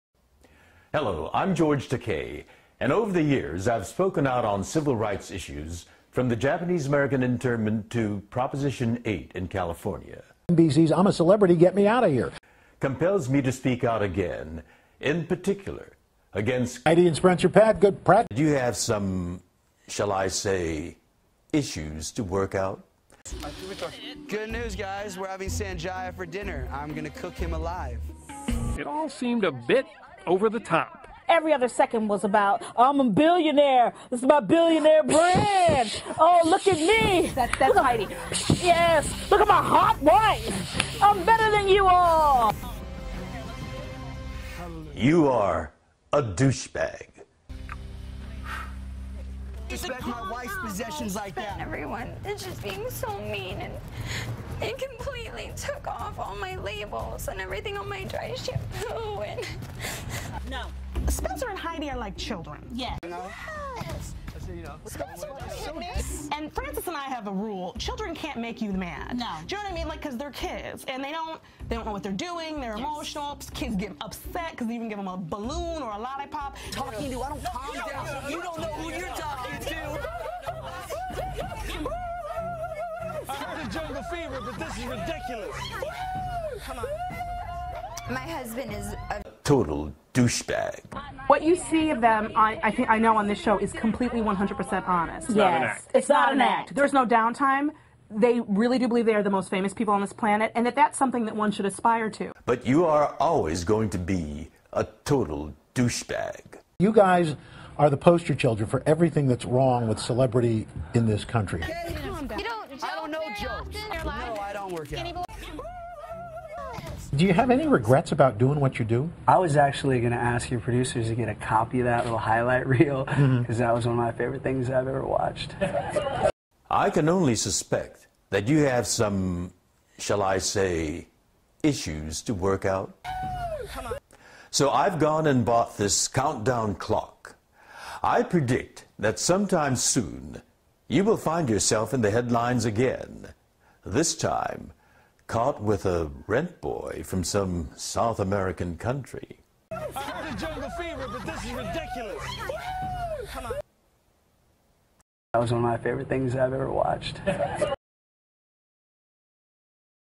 George Takei Calls Spencer Pratt a Douchebag
Heidi and Spencer Pratt truely are what is wrong with celebrities today..or should i say, "celebrities". They have no talent and make themselves famous by being errogant and calling attention to themselves. On another note, George Takei is my hero for calling out the actual Arkansas teacher that he made the video about.

its a little longer and gives Pratt more attention than I meant to and MUCH more than he deserves (considering he deserves to be drop-kicked into the ocean) but i still think its pretty funny. You do have to take it from a more general sense of his idiocy instead of reading it line for line.